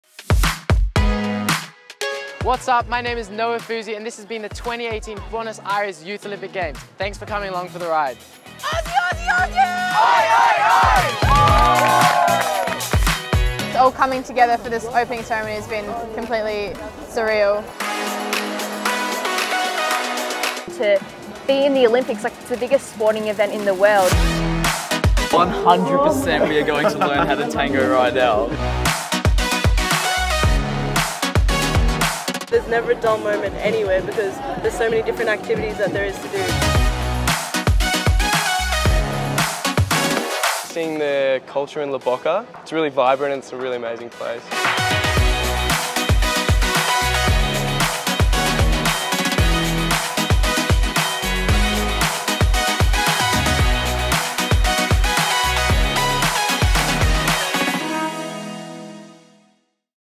Noah on the Road Ep 6 - Closing Ceremony
I was commissioned to make 6 x 60 second Instagram features for official Team Australia partner, Swisse Vitamins. We took professional skateboarder Noah Fuzi on the road to the 2018 Buenos Aires Youth Olympic Games following the journey of the Team Australian athletes for a look at life outside of the event.